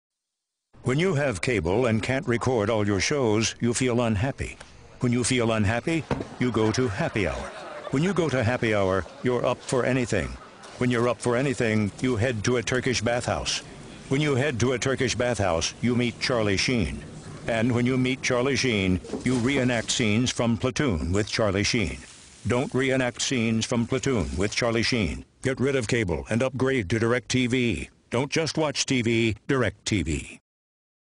CannesLions 2012 Gold Film: PLATOON
PLATOON
DIRECTV
TELECOMMUNICATIONS
GREY NEW YORK, USA
Advertiser DIRECTV
Product TELECOMMUNICATIONS
Entrant GREY NEW YORK, USA
5 of 6 Campaign
Type of Entry: Product & Service
Category: Commercial Public Services
Title: PLATOON
Advertiser/Client: DIRECTV
Product/Service: TELECOMMUNICATIONS
Entrant Company: GREY NEW YORK, USA
Advertising Agency: GREY NEW YORK, USA
CREATIVE CREDITS
Name Position
Tor Myhren (President/Cco) Chief Creative Officer
Dan Kelleher Executive Creative Director
Doug Fallon/Steven Fogel Creative Director
Steven Fogel Copywriter
Doug Fallon Art Director
Andrew Chinich/Lindsay Myers Agency Producer
Beth Culley/Kristen Stahl Account Supervisor
Jeff Mcdougall Producer
Tom Kuntz Director
Gavin Cutler/Mackenzie Cutler/Nick Divers/Valerie Sachs Editor
Sam Shaffer/Mackenzie Cutler Sound Design/Arrangement
Method Ny Post Production
Adam Clark/Josh Mandell Account Manager
Emmanuel Lubezki Director Of Photography
Project Manager: Joanne Peters Other Credits
Brief Explanation
See what happens when you have cable... upgrade to DirecTV. Don't reenact scenes from Platoon with Charlie Sheen!